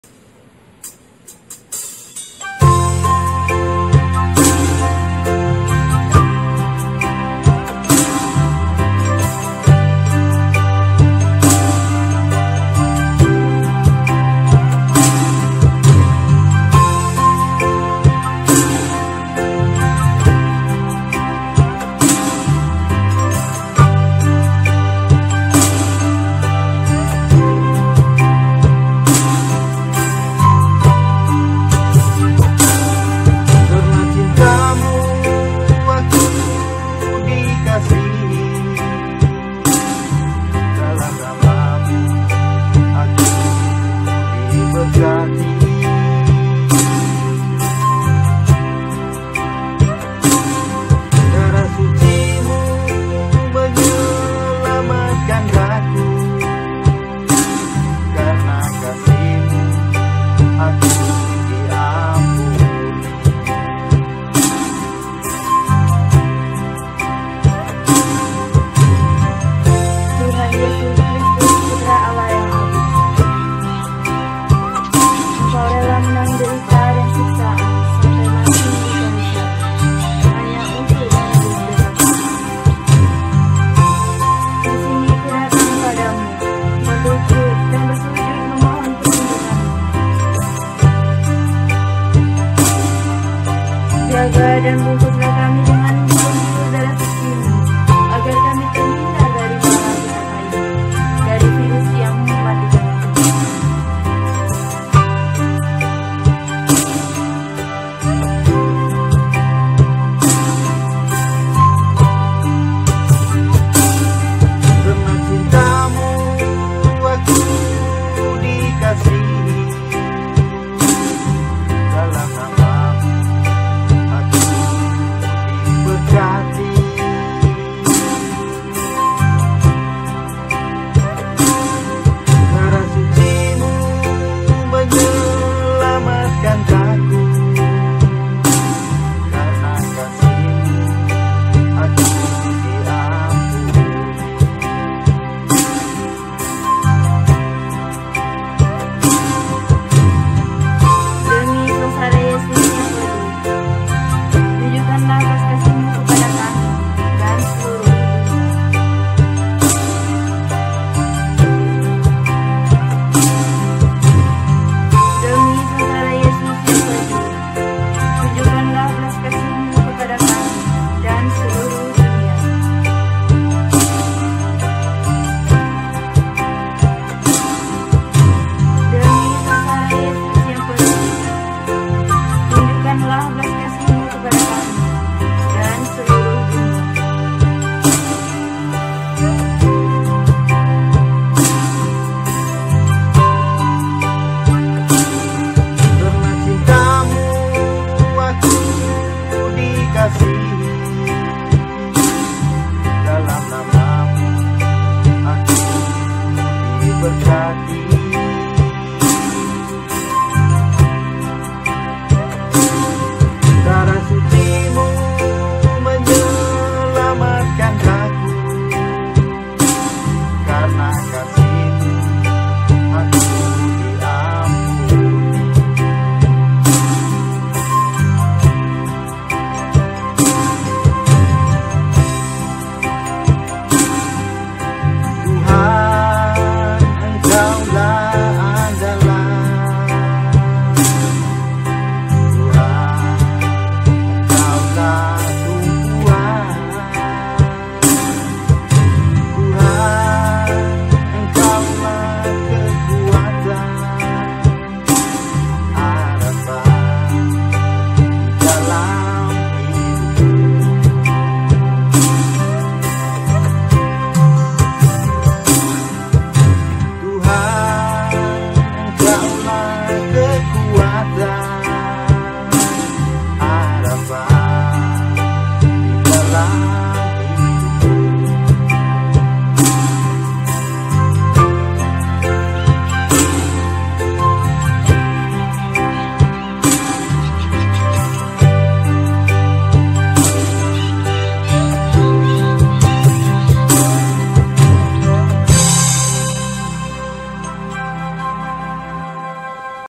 Tempat Ziarah umat Kristen Khatolik
Di sini merupakan tempat ziarah umat Katholik, peninggalan vietnam 1979~1996